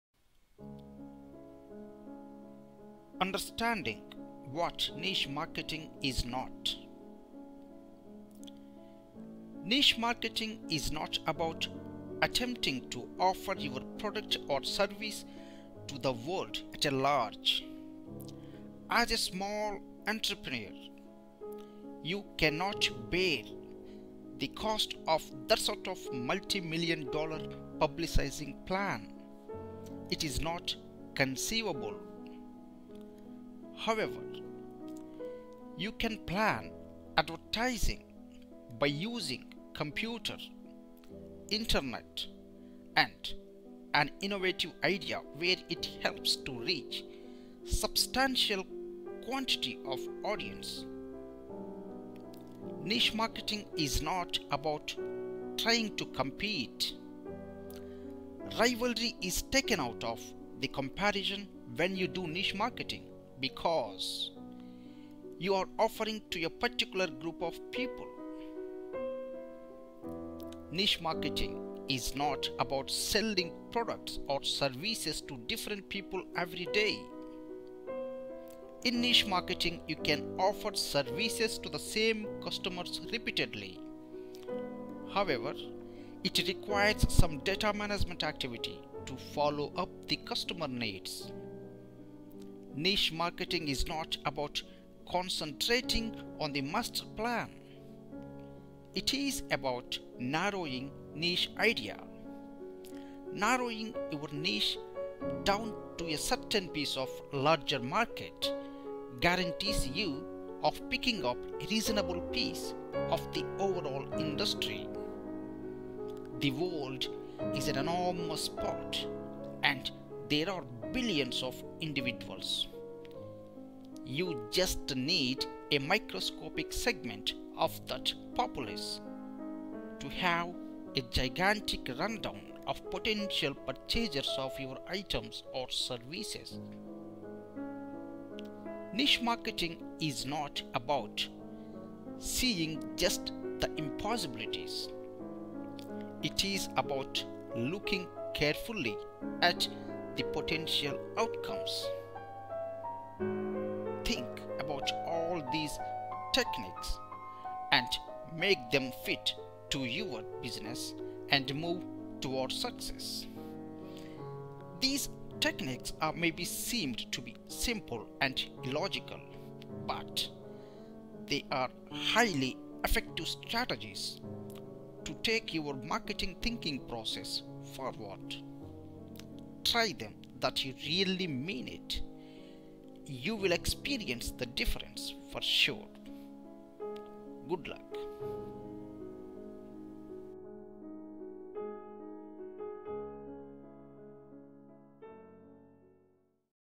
Understanding 'What niche marketing is not?'
These techniques may be seemed to be simple and illogical, but they are highly effective to take your marketing thinking process forward.  Try them and make them fit to your business.

You will experience the difference for sure.